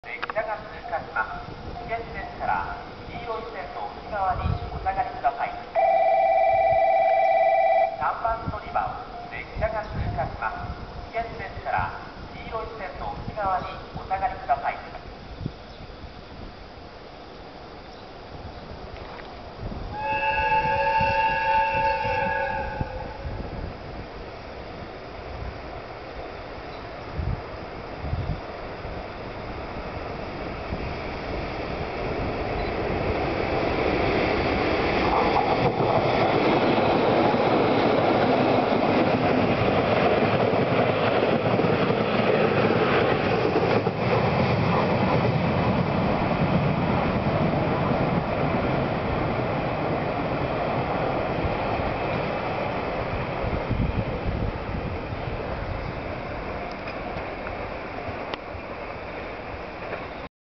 2010/9/26 臨時列車「ED76三重連明星号」通過@原田駅
2010/9/26　JR原田駅を通過する門司港(博多)行き臨時列車「ED76三重連明星号」(ED76_三重連+14系客車4両)を撮影しました。